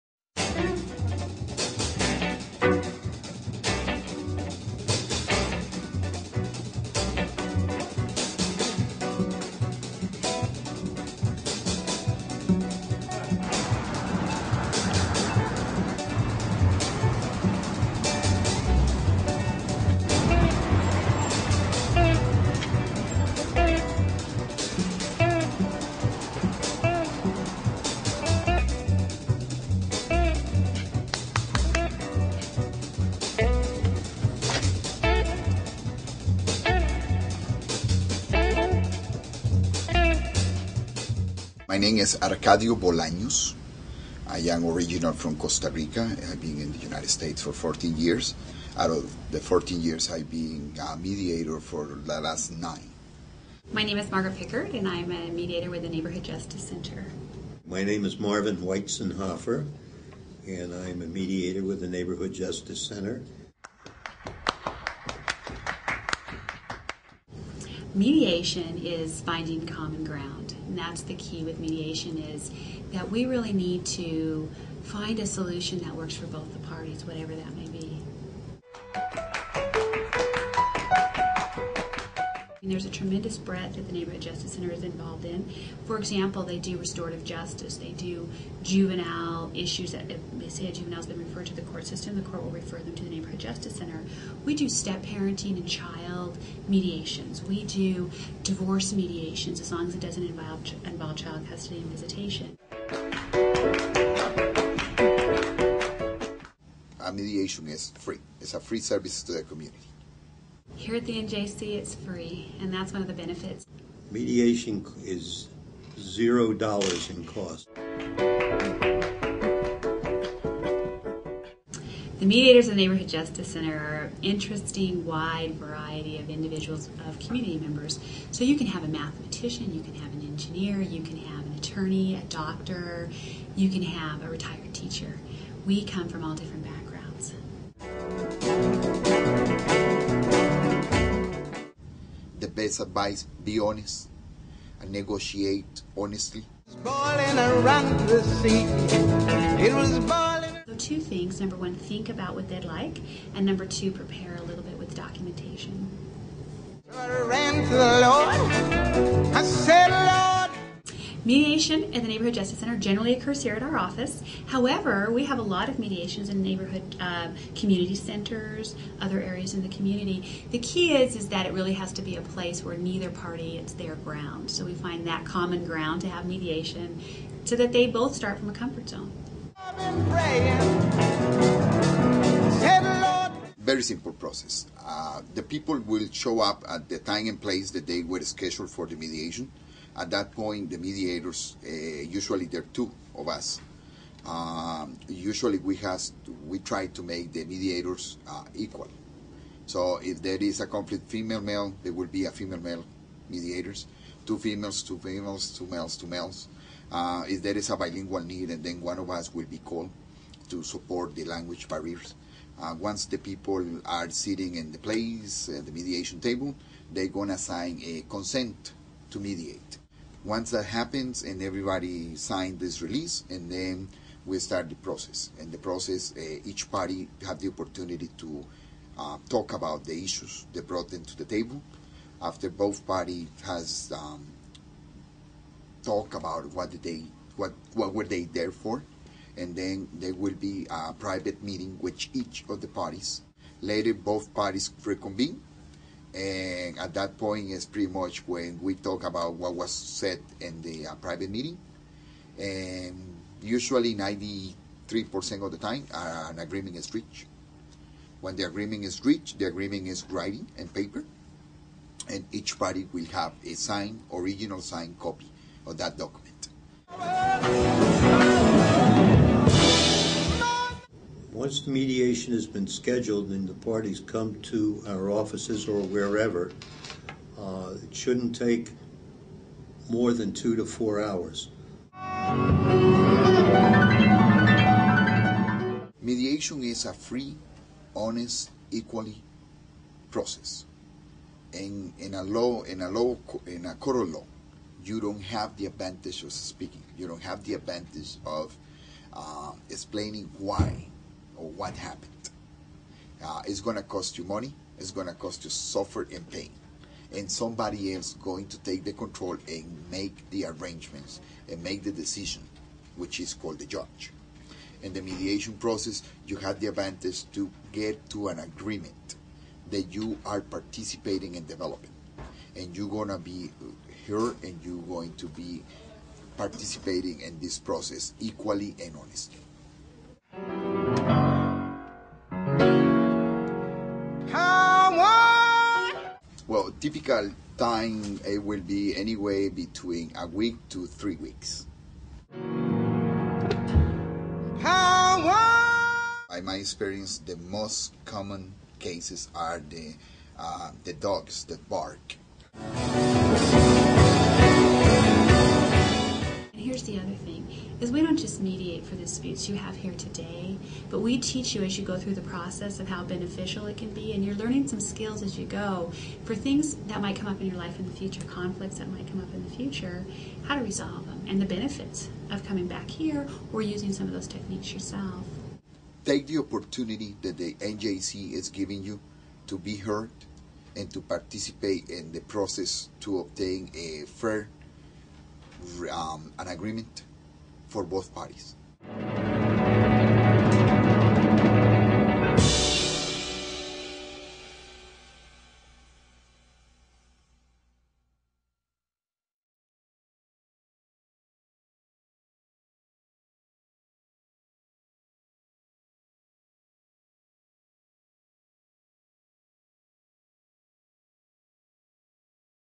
Community Mediation Programming - Overview (Las Vegas, NV)
Clark County, Nevada

This video features interviews from three volunteer mediators from NJC located in Las Vegas, NV.  The mediators answer many questions those considering or preparing for mediation may have.